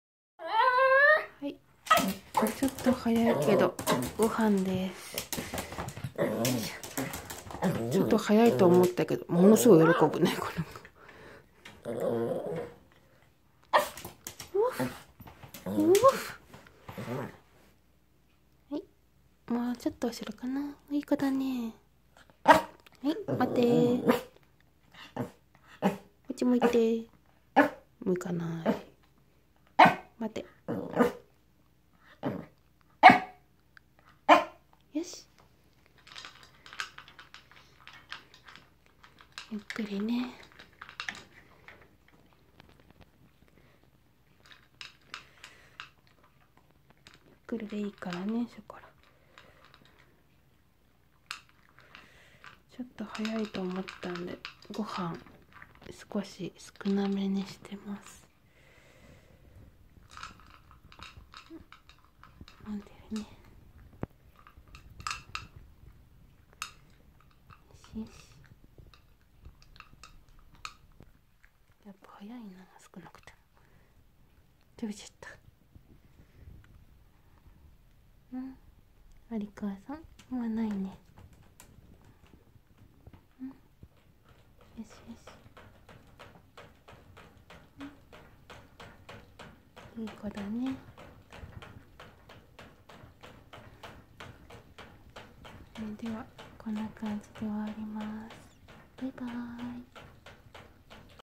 【Pet dog evening meal to eat】2/27 愛犬ショコラの夕ご飯
･夕ご飯です｡
少し早いですが､ご飯にしました｡
それにしても食欲あるね｡
ホント元気!!∪・ω・∪ﾜﾝｯ★